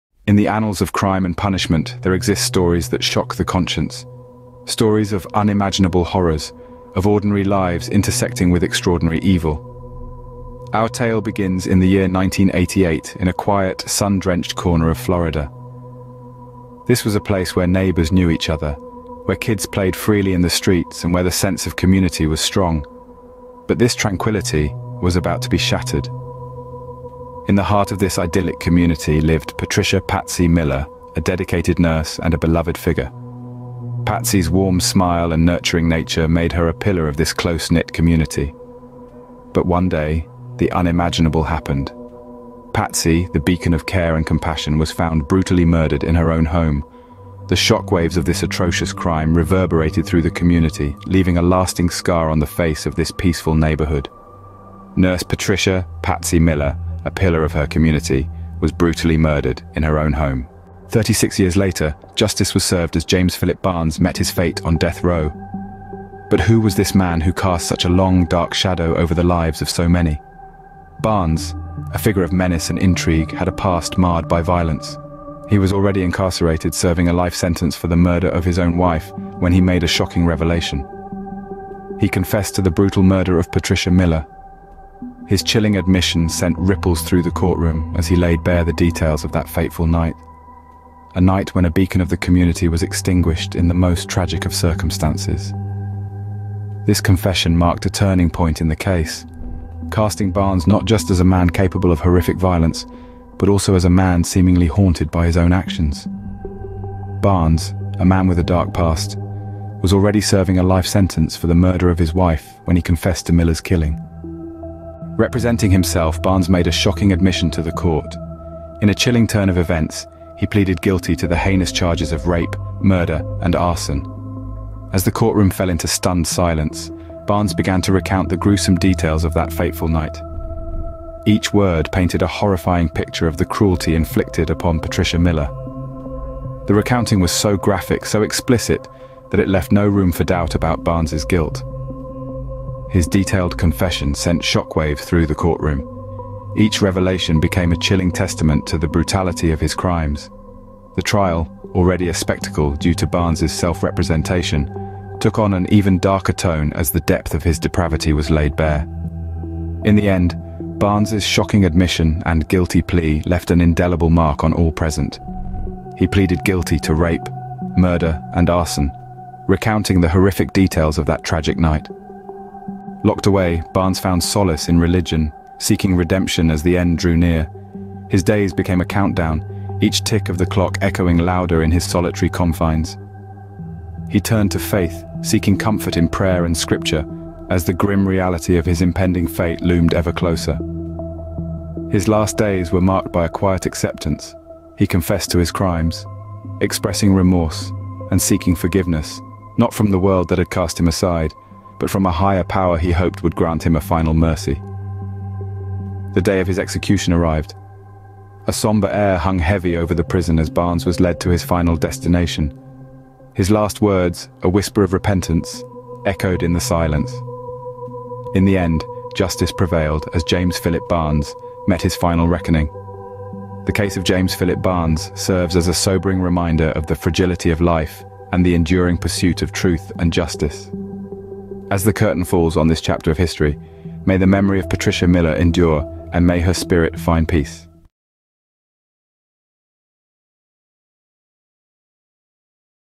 Silent Execution: The Tragic Case of James Phillip Barnes Death Row Documentary
Description:
Enter the chilling world of death row as we unravel the harrowing tale of James Phillip Barnes, a man whose crimes shocked a community to its core. From the brutal murder of nurse Patricia Miller to his haunting confession, Barnes' story is one of darkness and redemption. Join us as we explore the depths of human nature, the pursuit of justice, and the quest for closure in 'Silent Execution: The Tragic Case of James Phillip Barnes.' TrueCrime DeathRow JusticeServed"